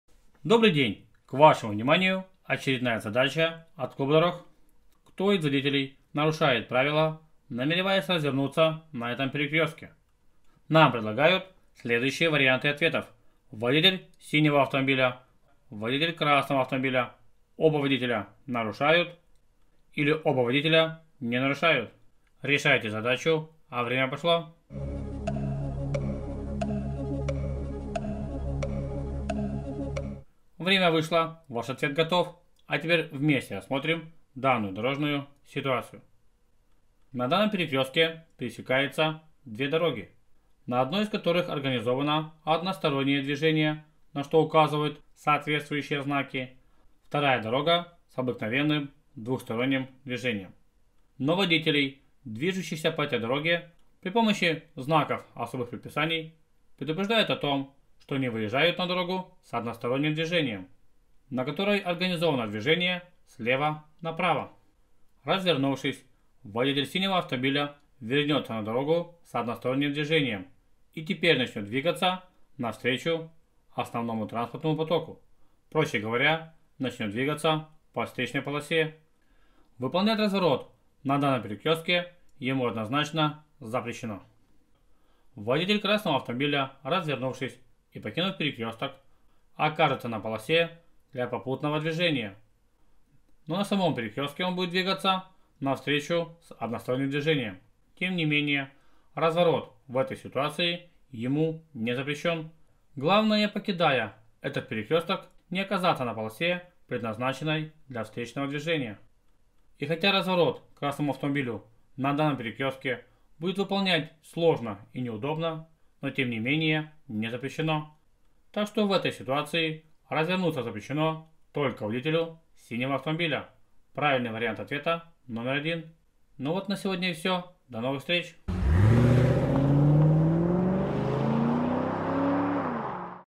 Кому из водителей запрещено выполнять разворот в данной ситуации?
Дорога, полосы, знаки и разметка, все эти элементы устанавливают свои предписания и ограничения при выборе разрешенной траектории для движения. Данная задача поможет вспомнить особенности выбора траекторий движения, в некоторых ситуациях.
Канал «Клуб Дорог» помогает выучить или вспомнить правила дорожного движения. Рассказывает про новости интересные для водителей, настоящих и будущих.